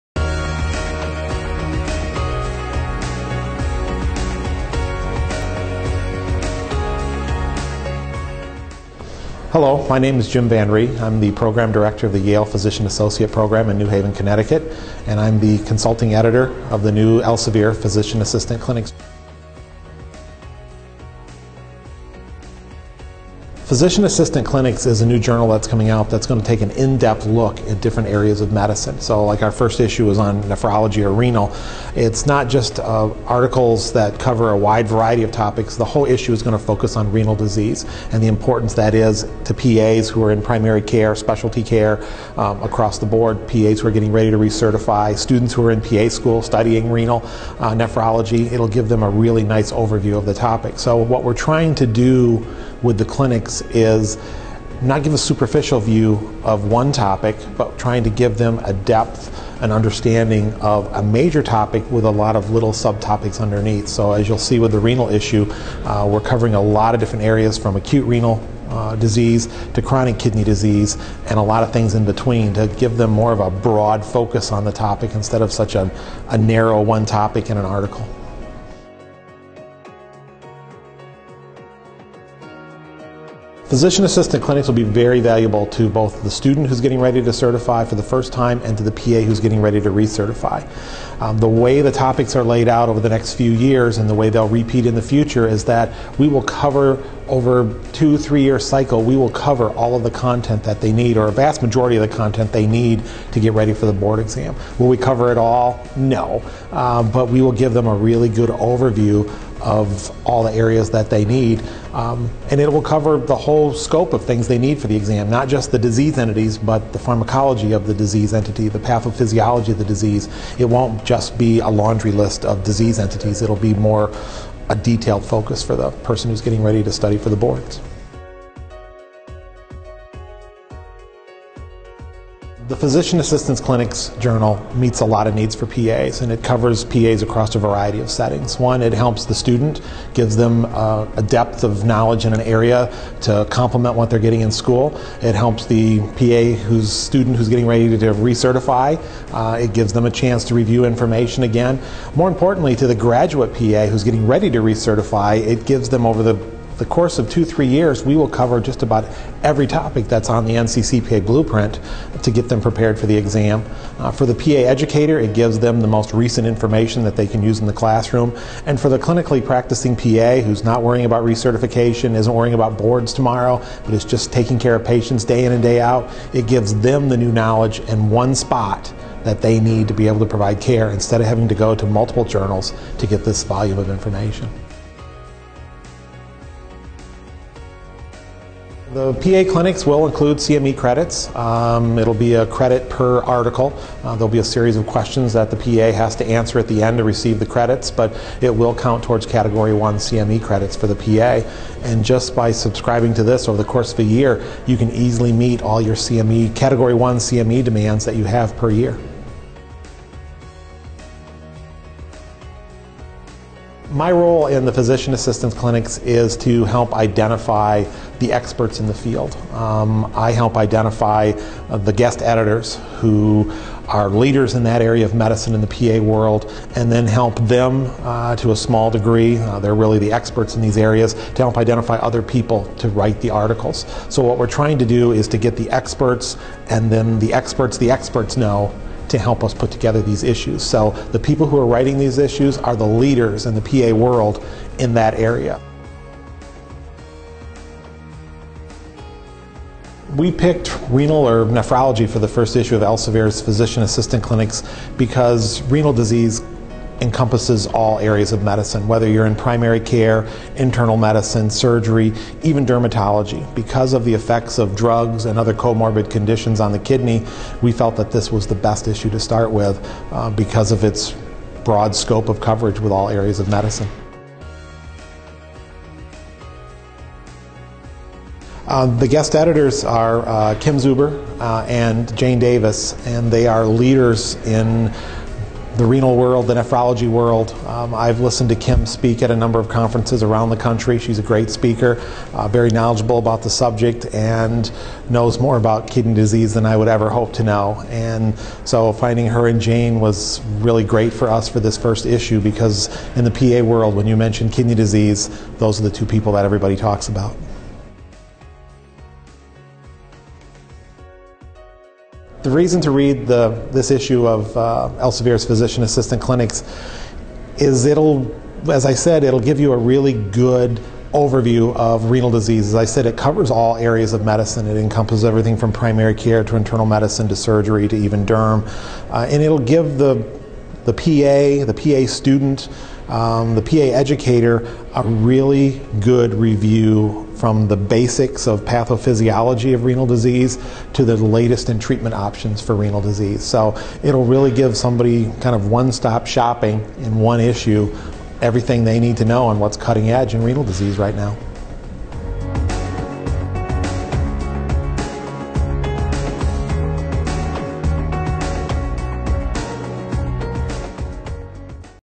NEW Physician Assistant Clinics
Consulting Editor of Physician Assistant Clinics, James Van Rhee, MS, PA-C, talks about the new clinical review publication written by PAs for PAs.